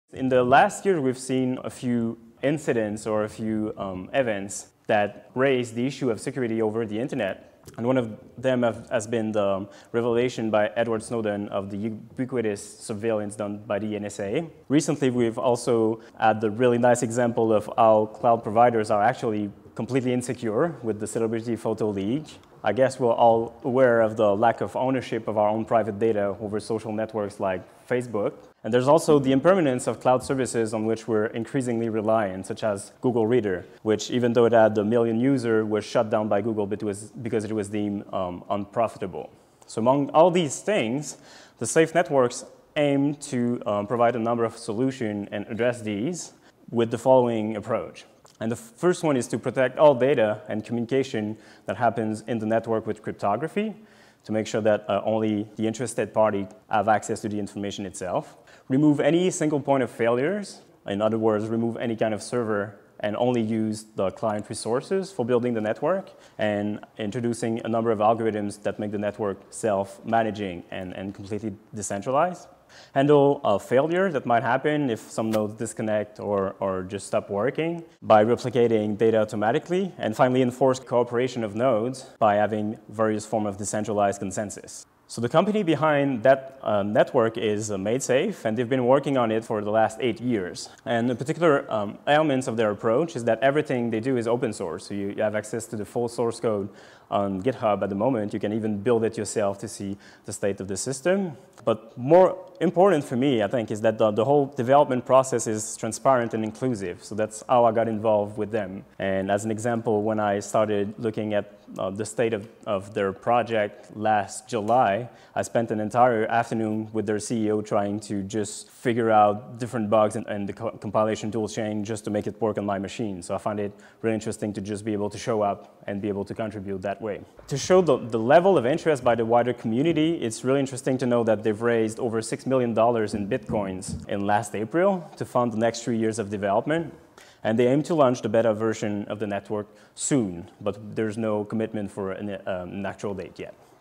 1.1. What is the SAFE Network?
The System Docs book provides a comprehensive introduction to MaidSafe and Project SAFE. This is an excellent starting point for those looking to 'dip their toe in the water’ and find out more about how the system works, getting involved with the network and the main platform components. While there are some technical sections, the bulk of the text is designed to be high level and accessible.